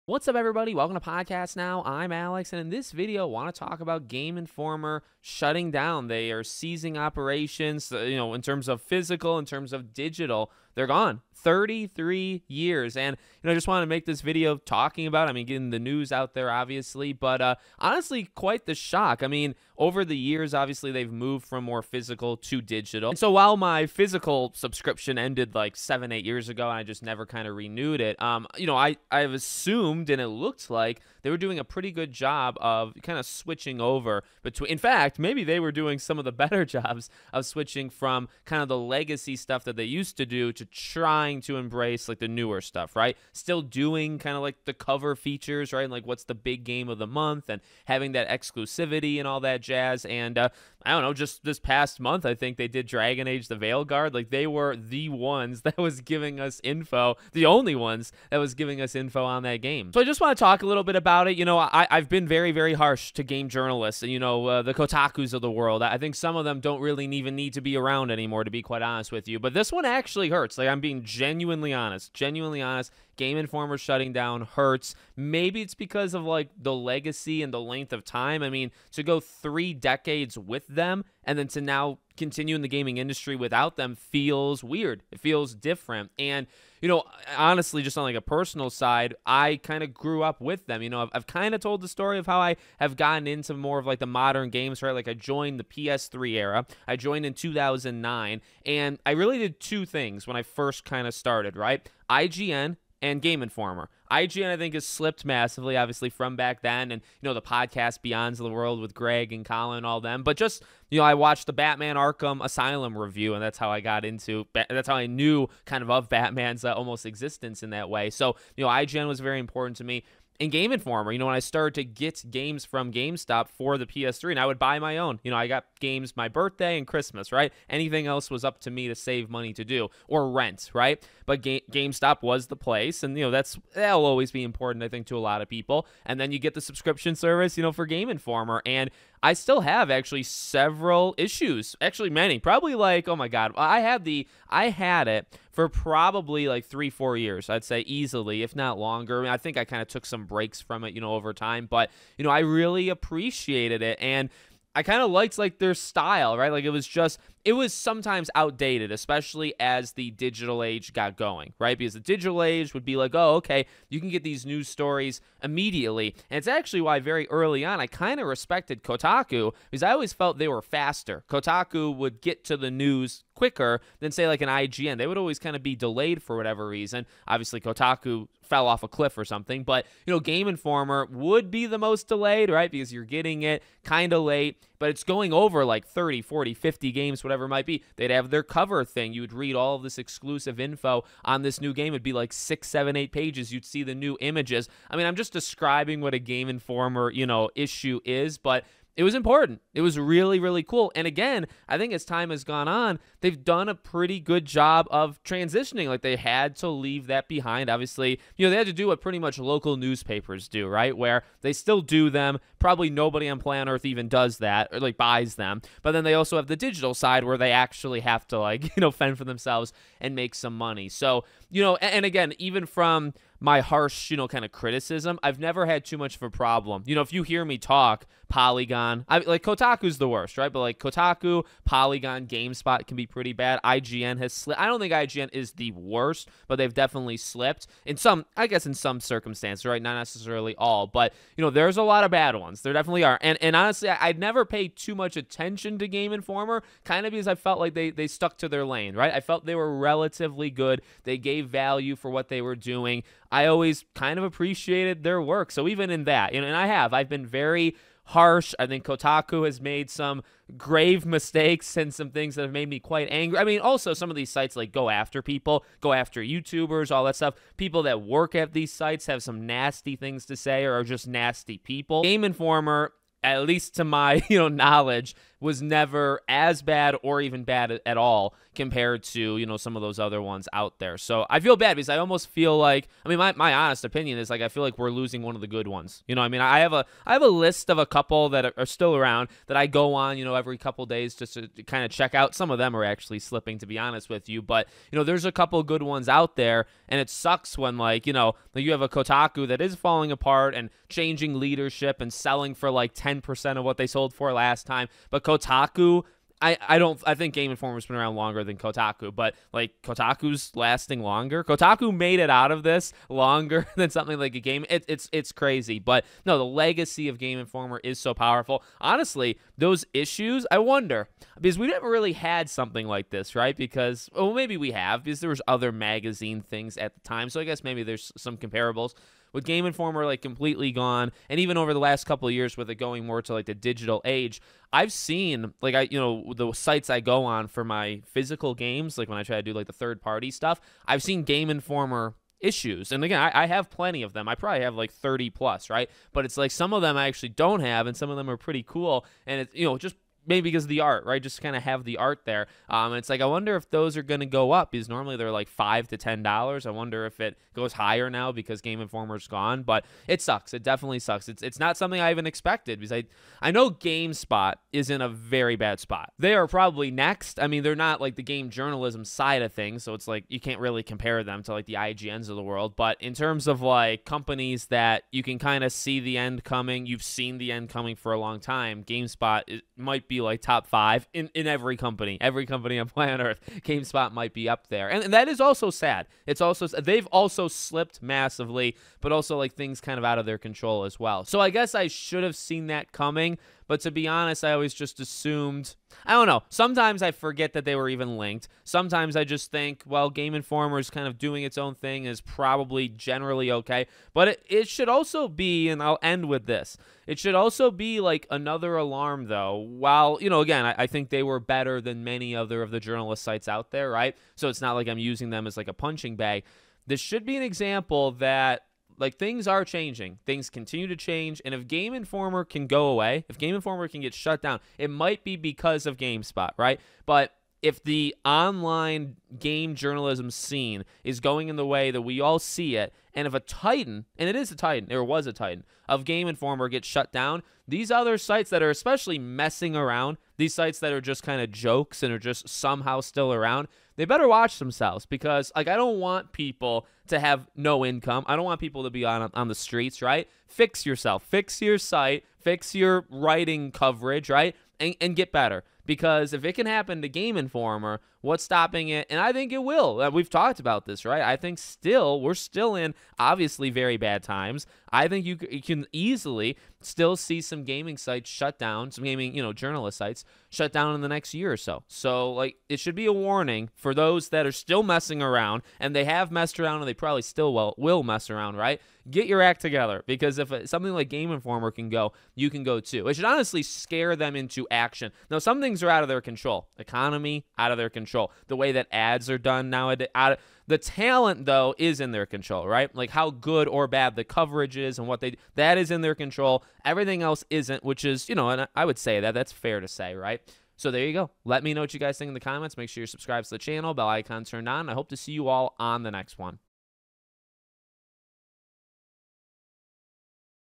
Game Informer Has Been SHUT DOWN | My Thoughts
Let's talk about Game Informer shutting down. It's a sad day, and honestly I thought they were one of the better ones.